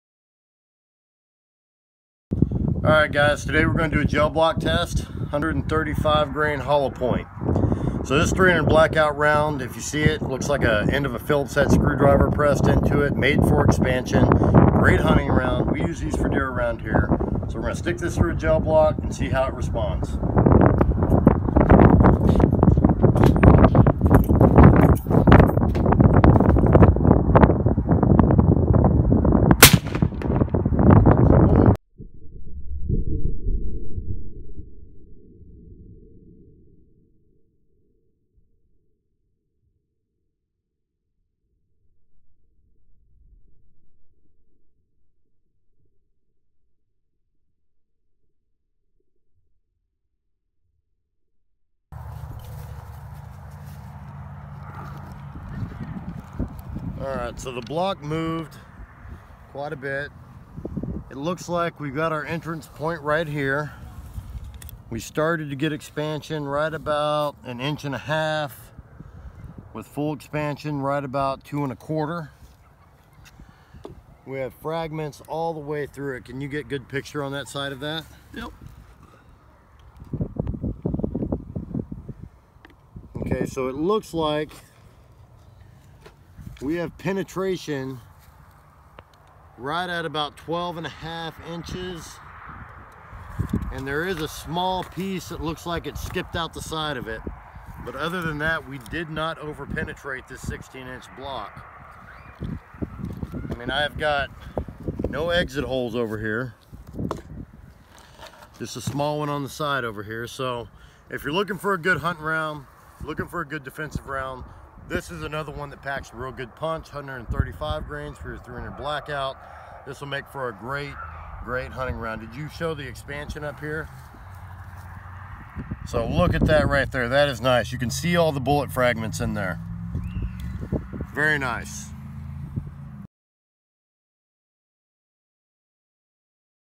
Ballistic Gel test 300 blk
135 grain HP
Mid America Munitions
Ballistic Gel test